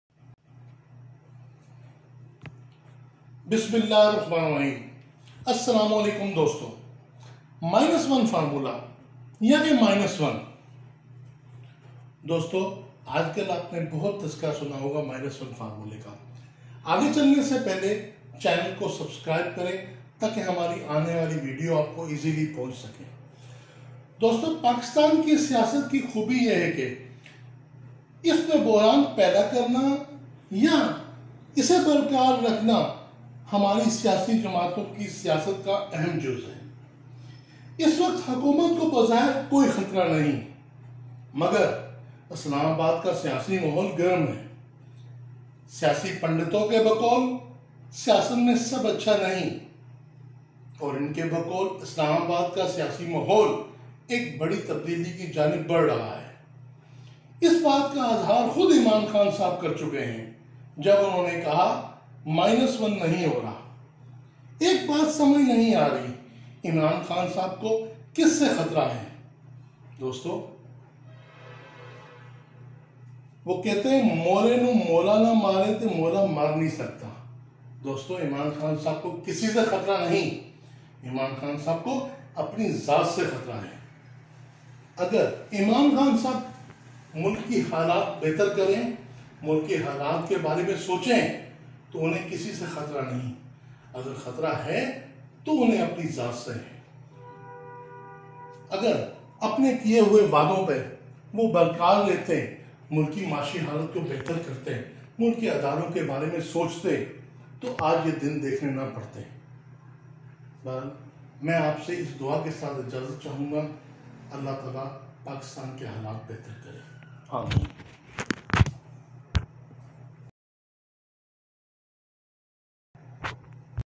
Minus One Formula
We know that Imran Khan knows more than anyone else that those who brought him to power are getting increasingly frustrated with his lack of performance. Despite having the full support of all the agencies and departments of the government, the PTI administration has miserably failed to resolve every crisis that it has faced. After nearly two years in power, the PM cannot get away with accusing the past governments or the mafias in the country for his failure to deliver.
The PM was no more bubbling with confidence.